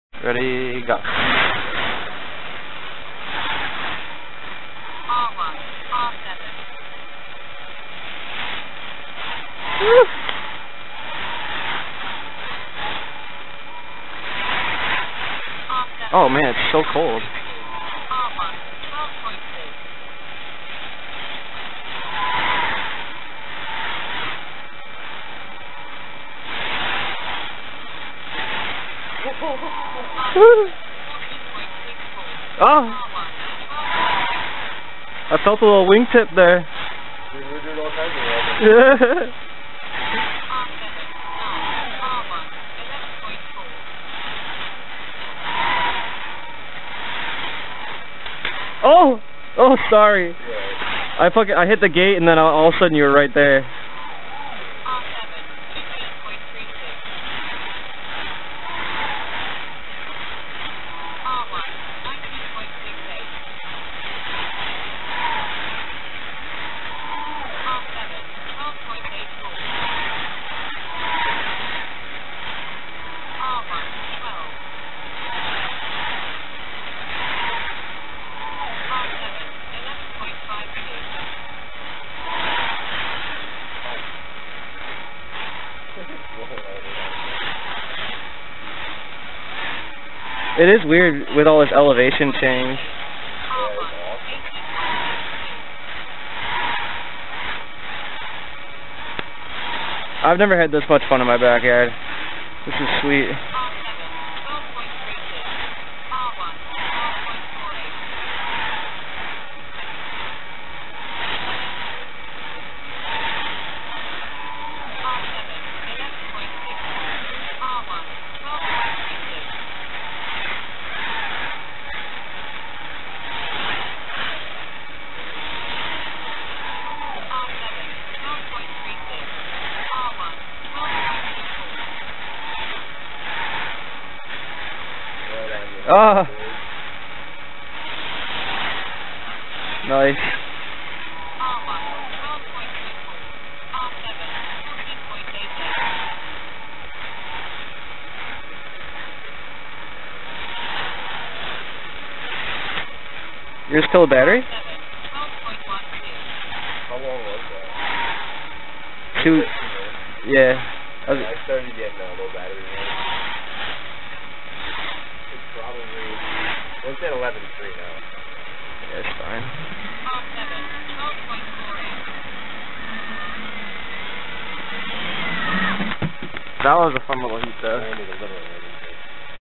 3s backyard duel
Daffer stopped by after work and we duked it out on 3s on the little backyard track. So much fun.